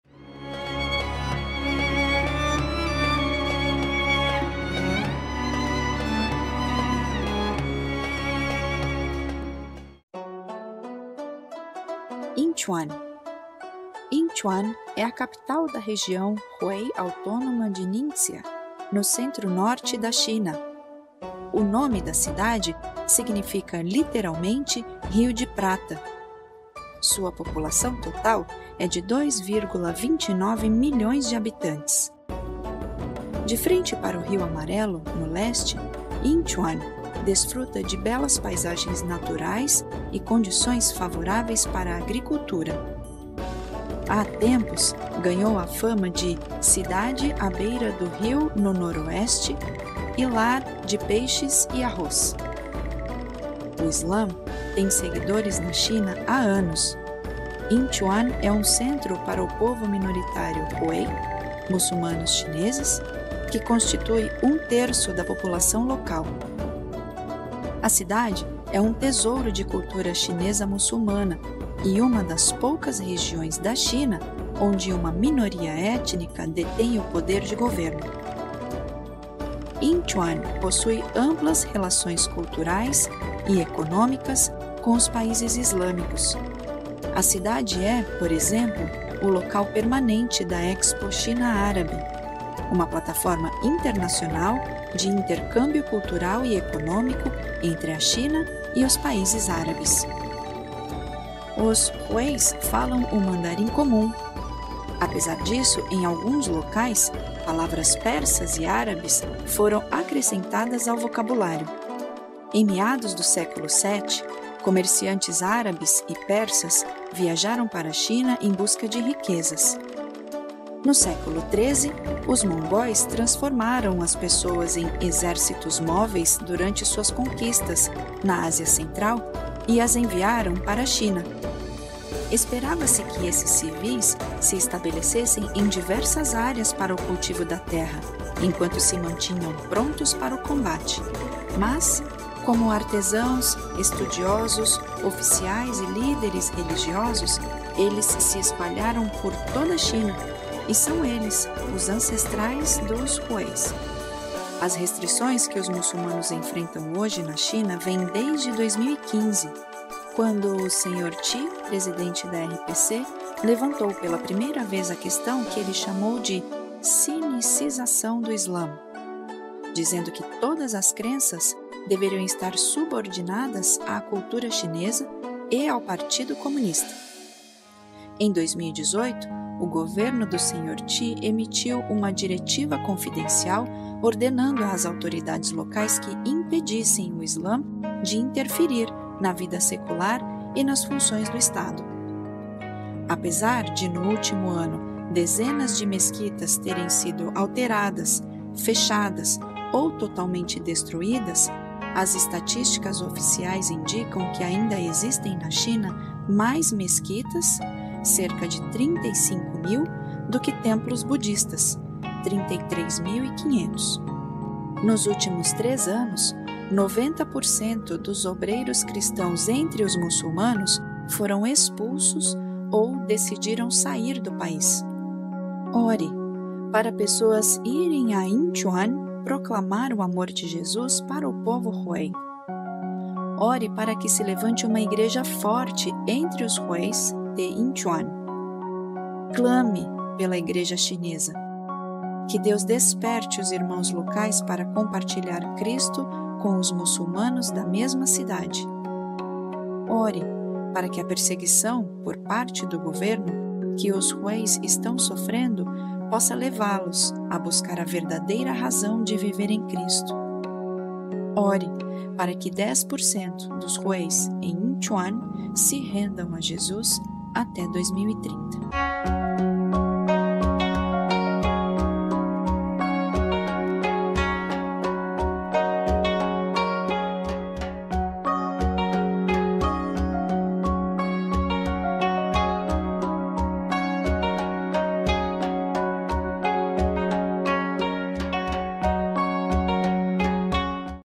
Rota do Sol #04 | Yinchuan, China
►  Yinchuan é a capital da Região Hui Autônoma de Ningxia, no centro norte da China. O nome da cidade significa, literalmente, "rio de prata''. Sua população total é de 2,29 milhões de habitantes. De frente para o rio Amarelo, no leste, Yinchuan desfruta de belas paisagens naturais e condições favoráveis para a agricultura.

VAMOS ORAR:

● Ore para pessoas irem a Yinchuan proclamar o amor de Jesus para o povo Hui;

● Ore para que se levante uma igreja forte entre os huis de Yinchuan;

● Clame pela igreja chinesa. Que Deus desperte os irmãos locais para compartilhar Cristo com os muçulmanos da mesma cidade;

● Ore para que a perseguição que os huis estão sofrendo, por parte do governo, possa levá-los a buscar a verdadeira razão de viver em Cristo;

● Ore para que 10% dos huis em Yinchuan se rendam a Jesus até 2030.

No ano de 2022, continuamos seguindo a RotadoSol em Intercessão pelo Mundo Muçulmano. Visitaremos 50 cidades estratégicas, em Oração pela Visão10/10: pedindo a Deus que 10% dos Muçulmanos dessas cidades conheçam Jesus nos próximos 10 anos.